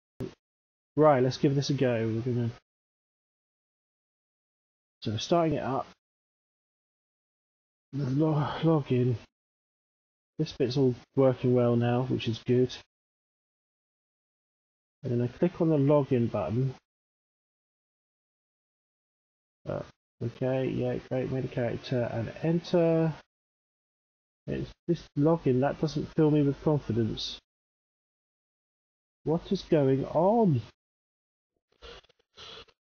problem logging in to atavism test server
login to character selection to blue login screen of fail